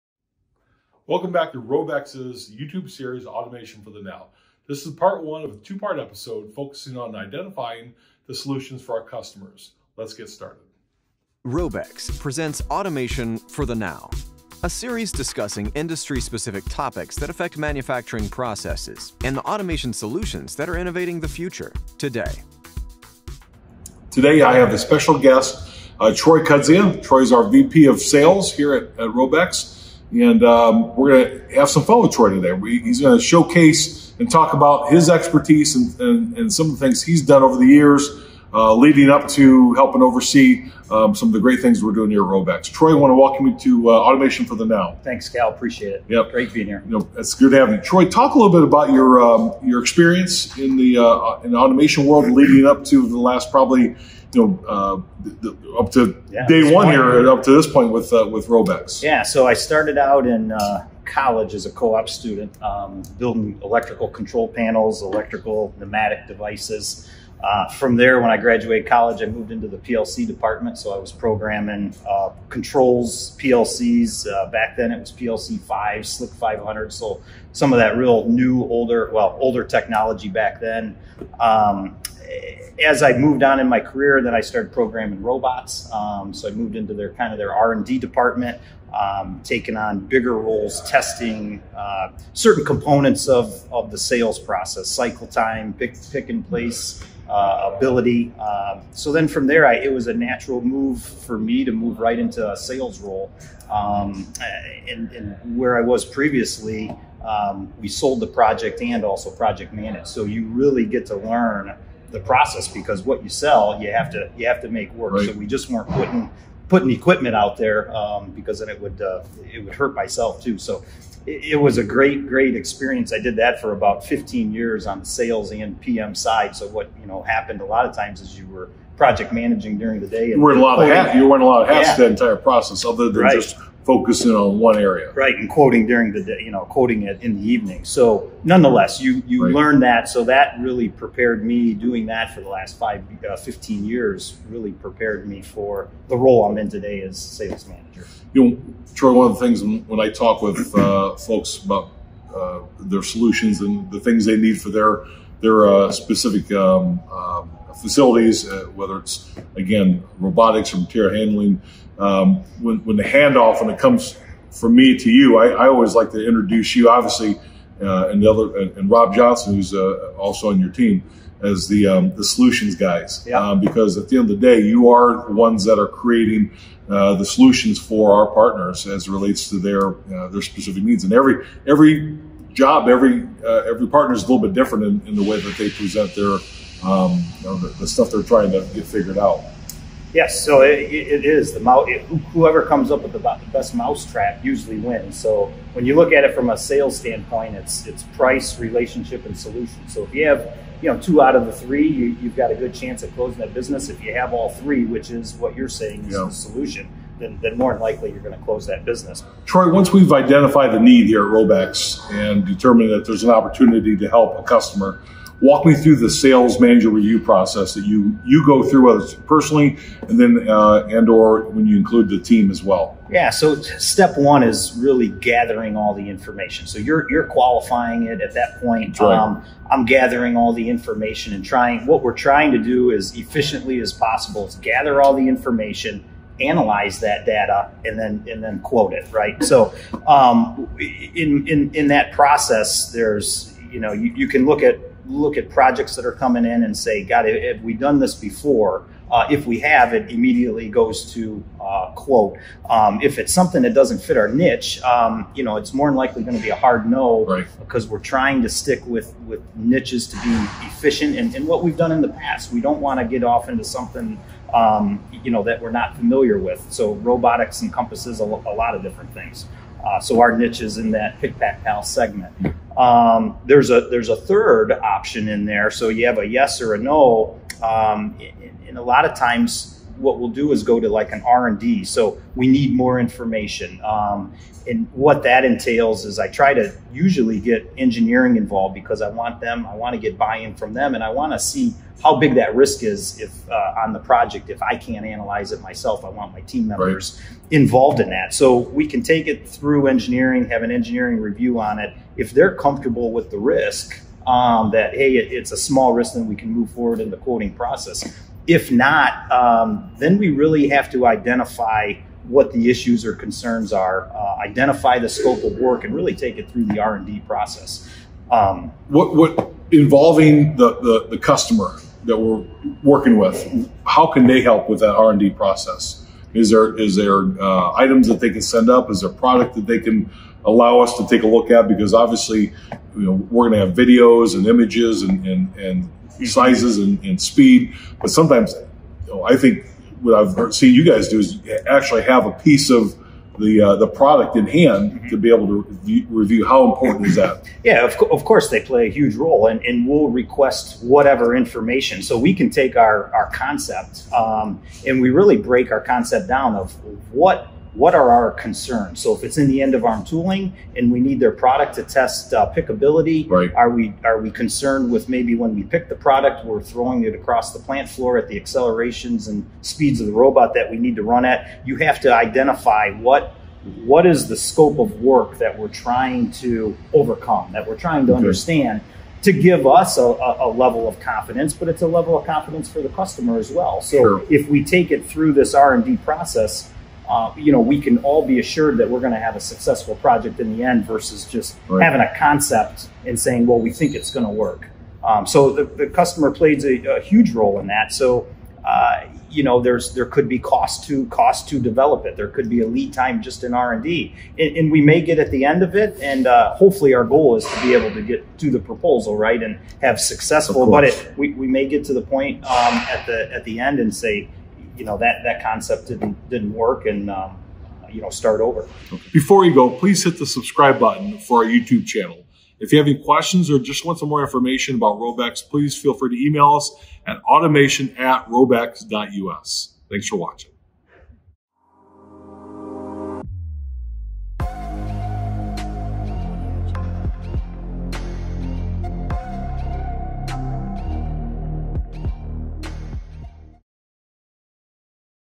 Automation for the NOW--Episode 2 | RōBEX
The 2nd episode of "Automation for the NOW" is LIVE and ready for viewing. There was so much good information in this one, we couldn't fit it all in one!

This is part of 1 of a 2-part episode discussing the next step in the RōBEX process - Identifying the Solution. Our host (and Director of Business Development) Cal Bowers is joined by VP of Sales Troy Kudzia.

Once we've identified the need for automation with our customer, a lot of detail goes into providing the right solution. Troy gives insight into how he & his team piece together all the necessary details to develop the best fit for our customer's manufacturing facility.

Learn more about our services and how we can help your business excel